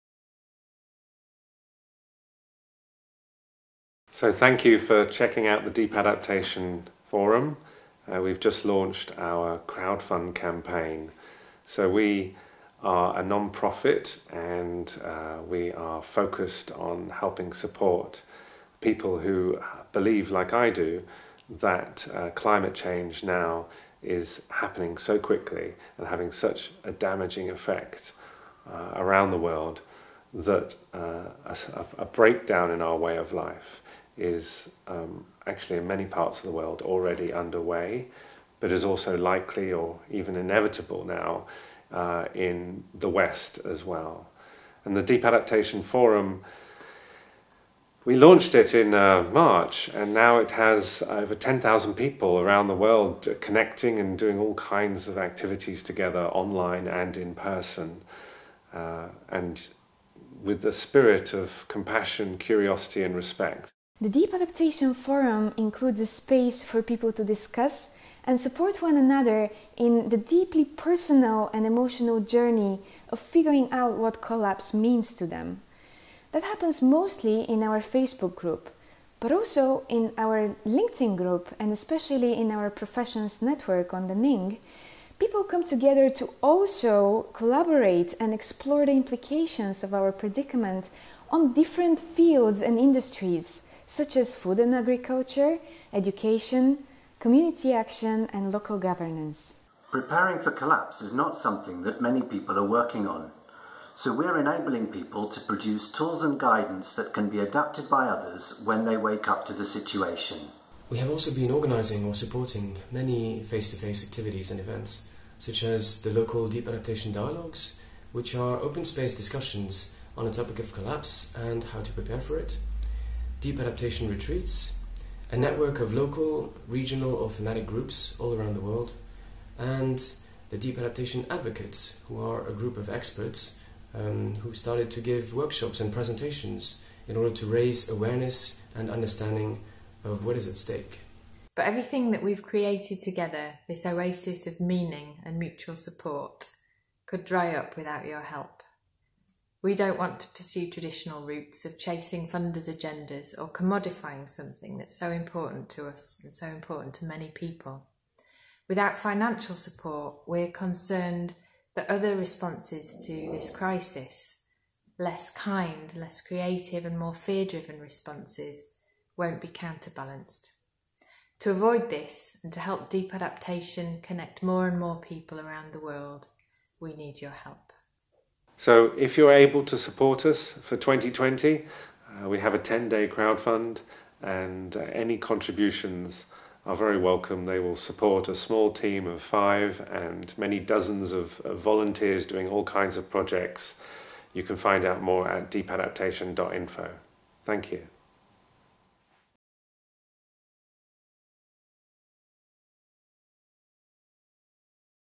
Deep Adaptation Crowdfund
With the invaluable help of our community, in the last 7 months, the Deep Adaptation Forum has organically grown to reach and impact the lives of more than 10,000 people. The driving force behind that growth has been simple - when faced with awareness of an environmental and social callapse due in our lifetimes, we need each other more than ever.

We need spaces to share about our emotional and spiritual journeys of embracing this awareness and making the necessary practical adjustments in our personal lives. We also need spaces to see what we can do about it, within our communities, within our fields of influence and professional expertise. At the Deep Adaptation Forum, it has been our mission to provide such spaces and nurture the constructive collaborative open discussion that is so deeply needed in our unprecedented times.

To continue serving our community in that way, however, we now ask for your help! We need GBP 32,000  (or approx. 41 280 USD) to fund our team, activities and projects in 2020. If you feel our work has brought value to you and you believe it’s important for it to continue, we are deeply grateful for any contribution you can make to our cause. Thank you!